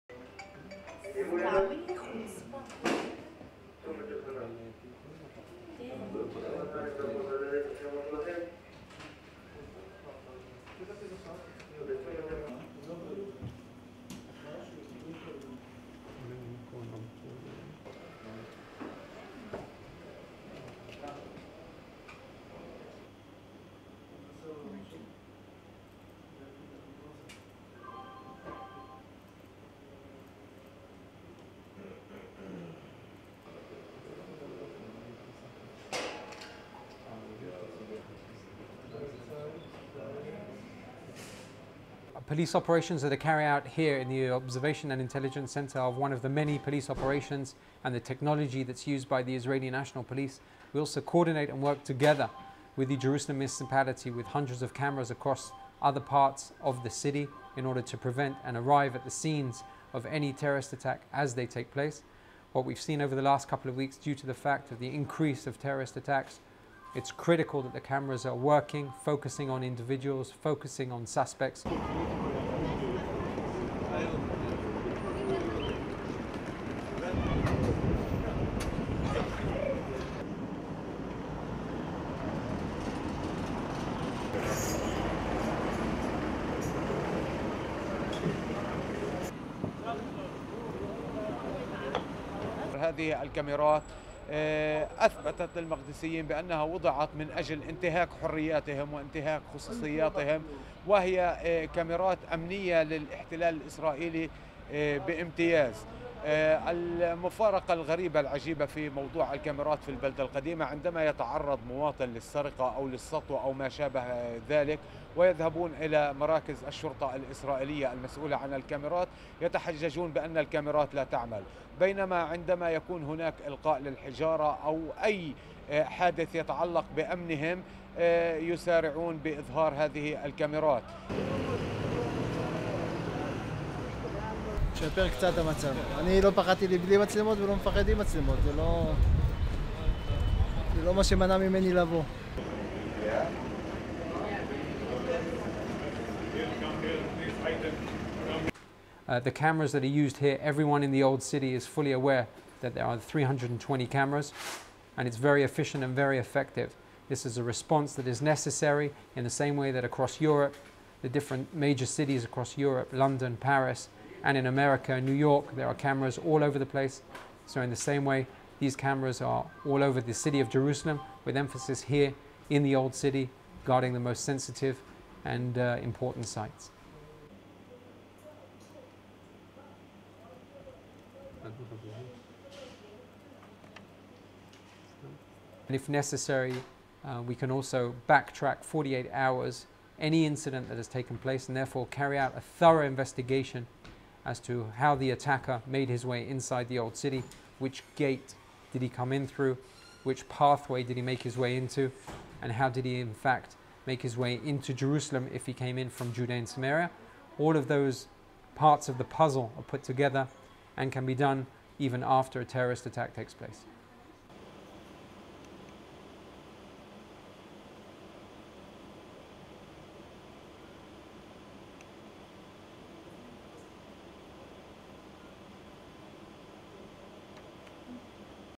Surveillance cameras monitor possible attacks
(18 Dec 2015) RESTRICTION SUMMARY: AP CLIENTS ONLY

Jerusalem - 17 December 2015
1. Pull out from screen showing people on the street captured by surveillance camera
2. Various of surveillance footage on screens
3. Police control room
4. Pan from screen showing surveillance footage to police officer watching screen
5. Various of police control room and surveillance footage
6. SOUNDBITE (English) Micky Rosenfeld, Israeli police spokesperson:
"Police operations that are carried out here in the observation and intelligence centre are one of the many police operations and the technology that is used by the Israeli national police. We also coordinate and work together with the Jerusalem municipality with hundreds of cameras across other parts of the city in order to prevent and arrive at the scenes of any terrorist attack as they take place. What we have seen over the last couple of weeks due to the fact of the increase of terrorist attacks, it is critical that the cameras are working, focusing on individuals, focusing on suspects."
7. Various of surveillance camera installed on Jaffa Gate in the Old City
8. Various of people in the Old City
9. Surveillance camera
10. SOUNDBITE (Arabic) Vox pop, Omar al-Shalabi, east Jerusalem resident:
"These cameras proved to the Jerusalemites (Palestinians) that they were placed in order to violate their freedom and privacy and they are security cameras for the Israeli occupation's benefit. What is strange with these cameras is that if the citizen is a victim of theft and he goes to the police station that is responsible for those cameras, the police argue that the cameras are not working, but if there is some stone throwing or any other accident related to their security, they quickly release camera footage."
11. Surveillance camera installed at the Old City
12. SOUNDBITE (Hebrew) Vardi, surname not provided, visitor from Tel Aviv:
"It improves the situation a bit, I was not afraid without cameras and am not afraid with cameras, this is not what prevented me from coming "
13. People in Old City
14. SOUNDBITE (English) Micky Rosenfeld, Israeli police spokesperson:
"The cameras that are used here, everyone in the old city is fully aware that they are on 320 cameras and it is very efficient and very effective. This is a response that is necessary in the same way that across Europe, the different major cities across Europe - London , Paris, and in America, in New York, there are cameras all over the place so in the same way these cameras are all over the city of Jerusalem, with emphasis here in the Old City, guarding the most sensitive and important sites."
15. Screen showing people on the street captured by surveillance camera
16. SOUNDBITE (English) Micky Rosenfeld, Israeli police spokesperson:
"If necessary we can also backtrack 48 hours any incident that has taken place and, therefore, carry out a thorough investigation as to how the attacker made his way inside the Old City, which gate did he come in through, which pathway did he make his way in to, and how did he in fact make his way into Jerusalem if he came in from Judea and Samaria (West Bank). All of those parts of the puzzle are put together and can be done even after a terrorist attack takes place."
17. Zoom in on surveillance camera footage
STORYLINE:
Jerusalem police plan to install additional surveillance cameras across the Old City to monitor possible attacks following an increase in violence between Israelis and Palestinians.
The police plans to deploy more cameras in 2016.